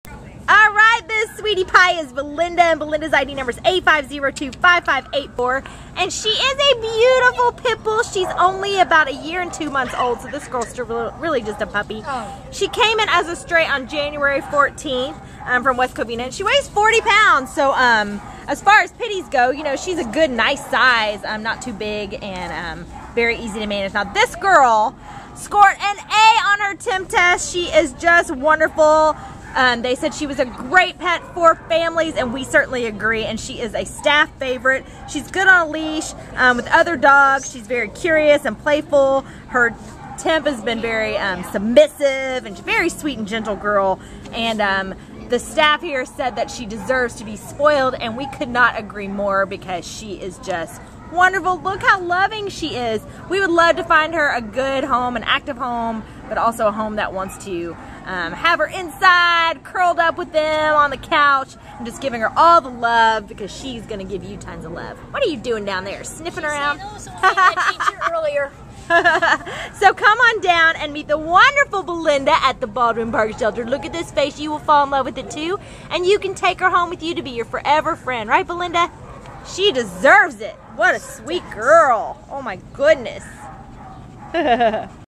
A5025584 Belinda | Pit Bull Mix Puppy
5025584 Belinda is a love-muffin of a 1-year-old blue-and-white female Pit Bull mix puppy who came to the Baldwin Park Animal Care Center on January 14th as a stray from West Covina. Weighing 40 lbs, Belinda passed her shelter-administered temperament test with flying colors and is a staff favorite! This sweet girl is good on leash and has the best personality ever. She is affectionate, gentle, and eager to please. Belinda is friendly and playful with other dogs. She’s also a pretty, pretty girl -- look at that sweet face! Belinda would be an outstanding indoor pet for anyone who wants a puppy to love, train, and spoil, and we think she’d be wonderful with kids.